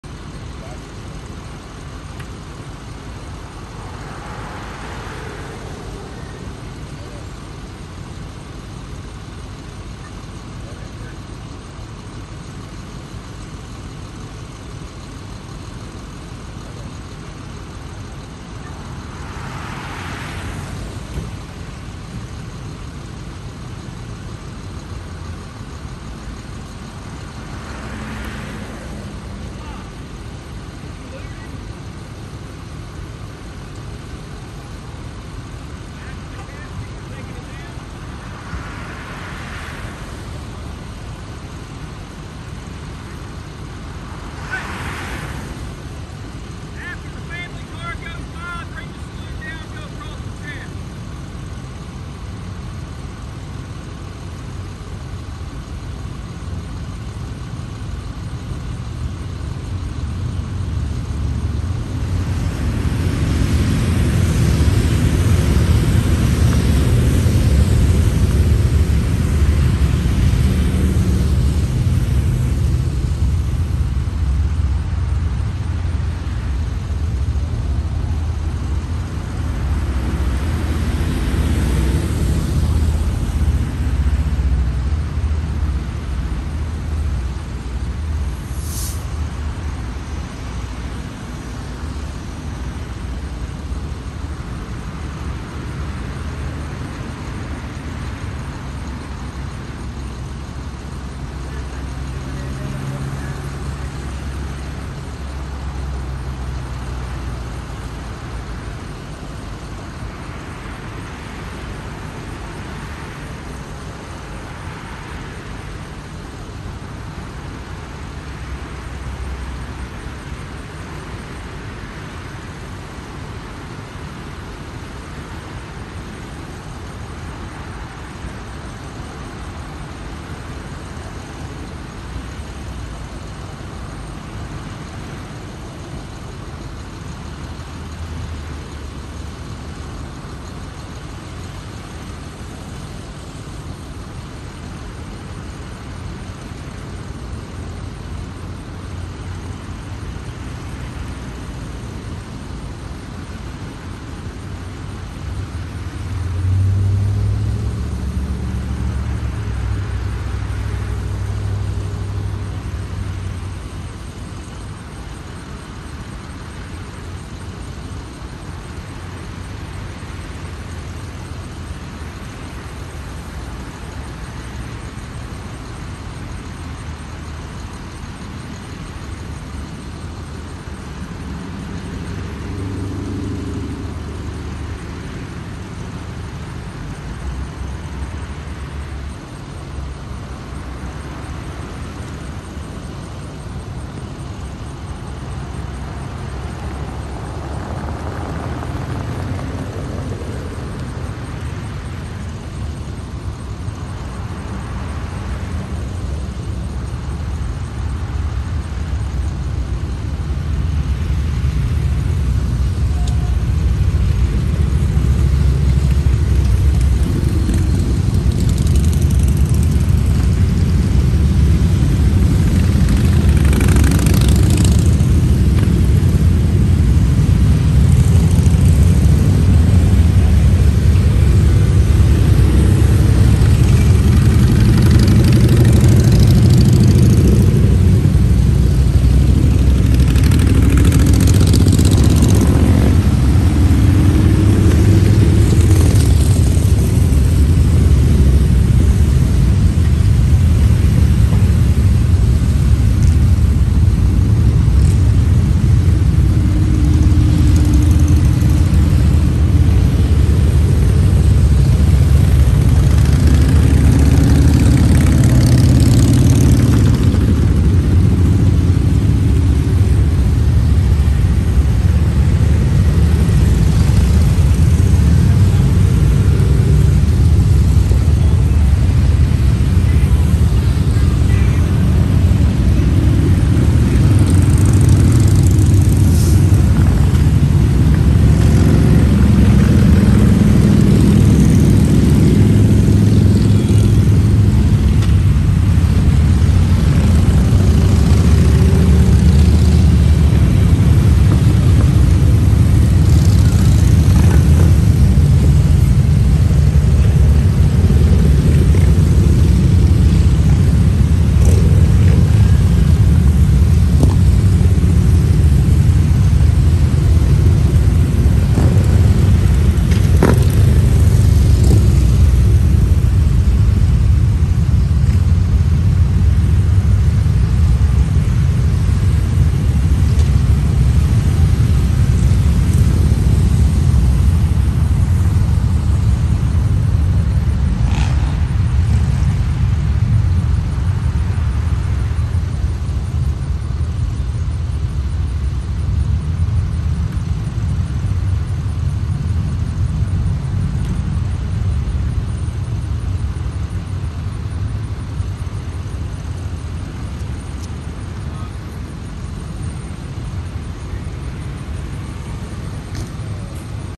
Hershel “Woody” Williams Funeral Escort
This was the funeral escort from Huntington to the WV state capital for Hershel "Woody" Williams on Saturday July 2 2022.
The funeral is scheduled for Sunday July 3rd.
Woody was our nation's last surviving WWII Medal of Honor recipient.
Woody lived here in Cabell County WV.
He passed away on June 29th.